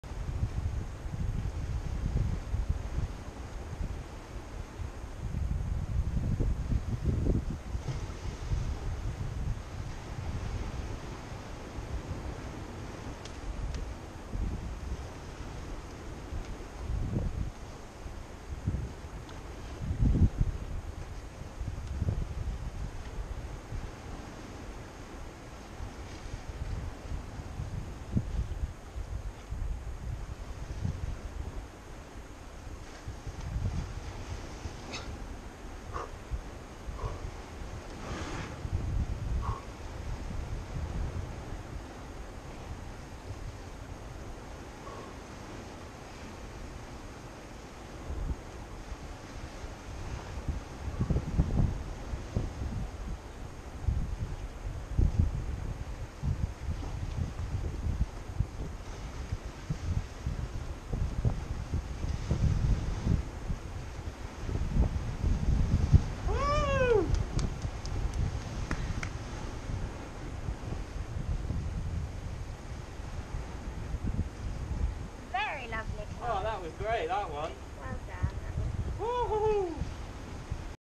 Undercooke Arete - Font 7A+ - St Bees
Same starting moves as Undercooke, but once established on the obvious crimps launch rightwards towards the arete and finish up this. This isn't in any guidebooks which is a shame because it climbs so well!